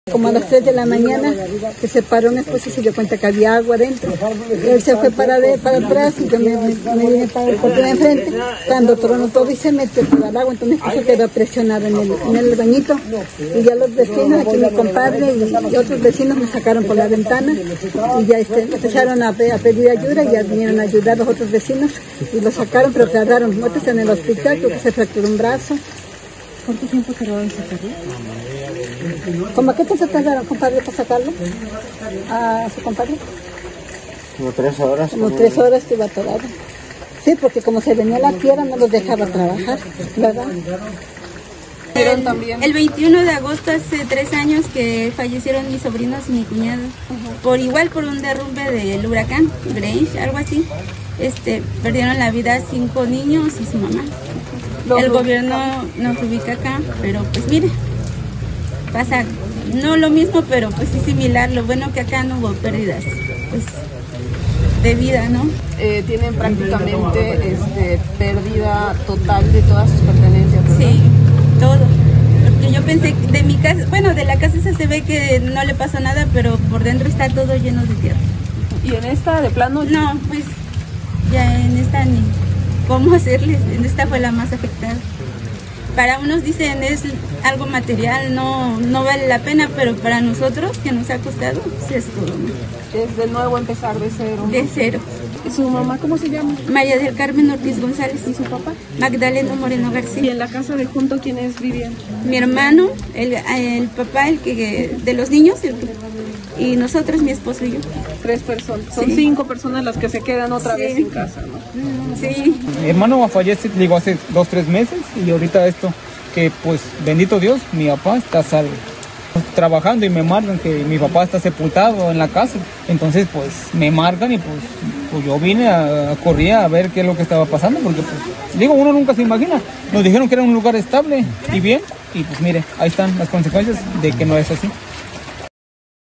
La tragedia persigue a los Moreno Ortiz
Hace tres años, nuera y cinco nietos murieron en Brisas de Sedeño. Los deudos fueron reubicados sobre avenida Murillo Vidal y hoy de nuevo, un alud de tierra los sepulta.